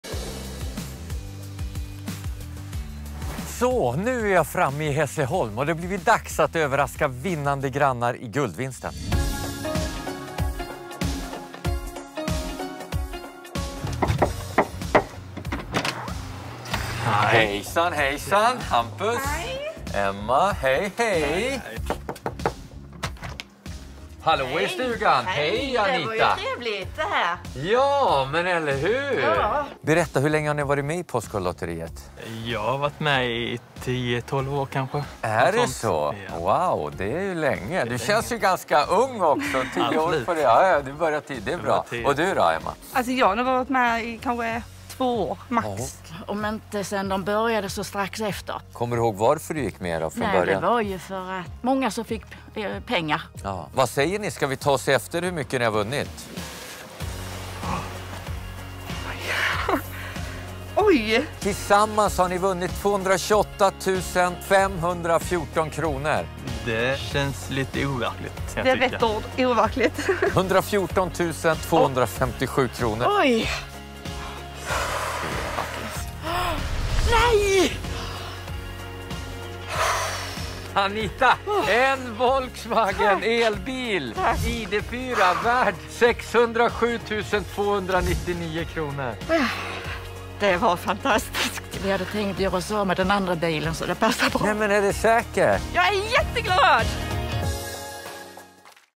Härliga vinnare i Hässleholm vinner stort i Postkodlotteriet
Möt de gulliga grannarna i Hässleholm, som överraskas av Jesper med vinstbesked. En glad dag helt enkelt! Stort grattis alla